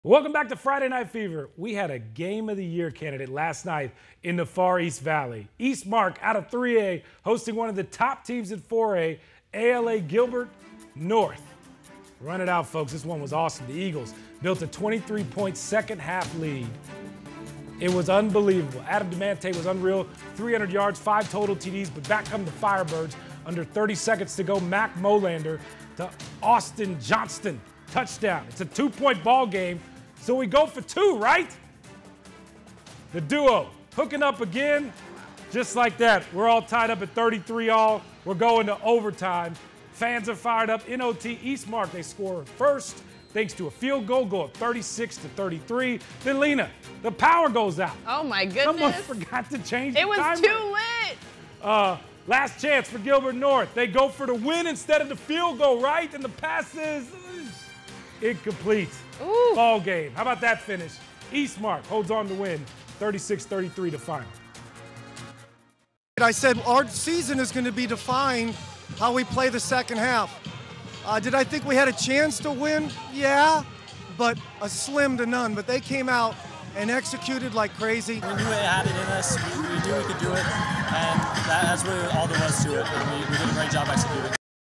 Eastmark defeats ALA-Gilbert North 36-33
Eastmark comeback to defeat ALA-Gilbert in OT on Thursday night.